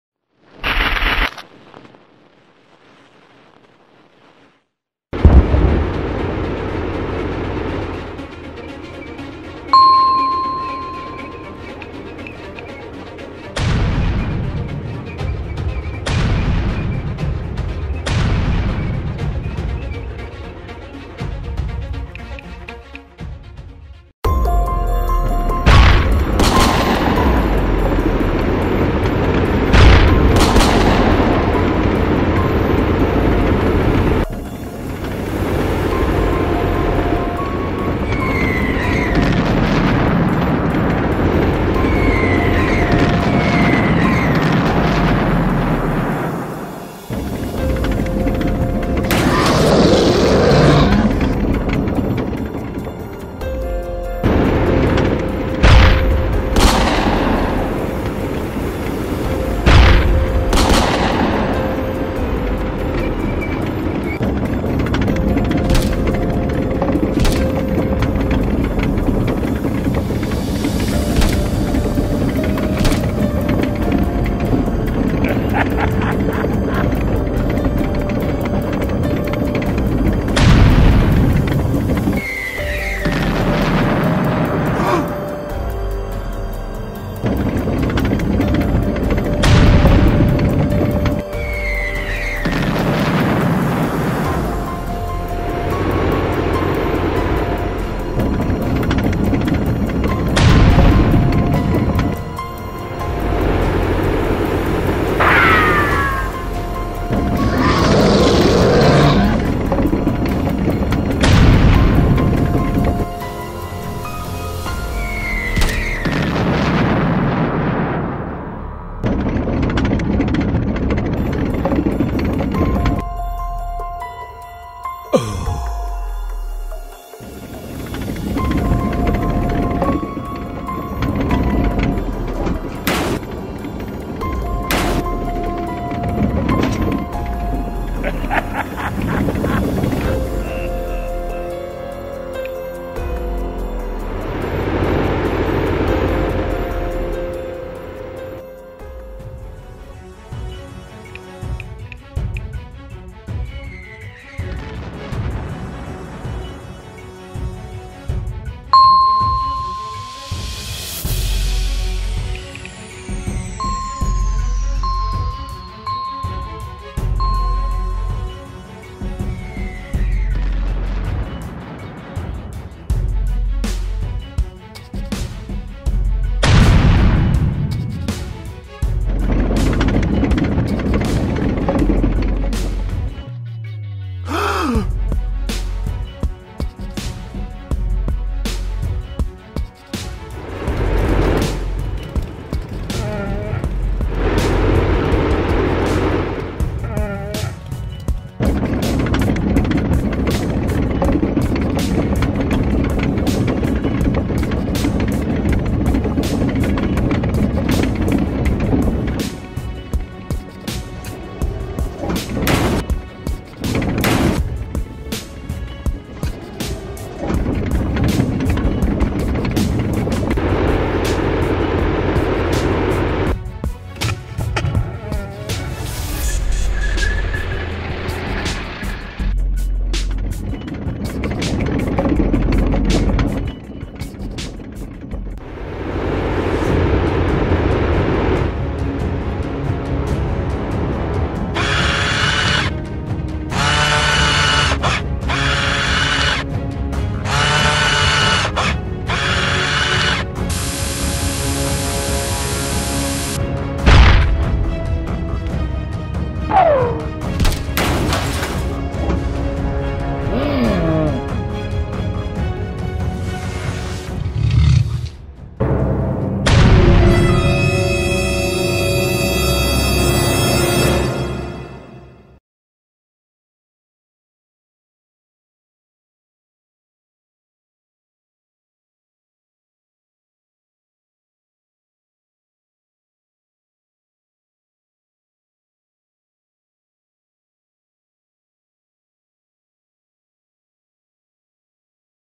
Vulnerable spot. Cartoons about tanks. Third episode
Description
Watch the third episode of the new cartoon about tanks, called "Weak Point".
The hero T-34-85 flees into a mysterious cave to an old tank, and he tells him his story. How an old tank met a monster and almost died, and the main one shows our hero how to destroy the leader of the enemies.
Rather watch the entire episode in its entirety, write comments and like it !

and put a bell so as not to miss the new series of cartoons about tanks.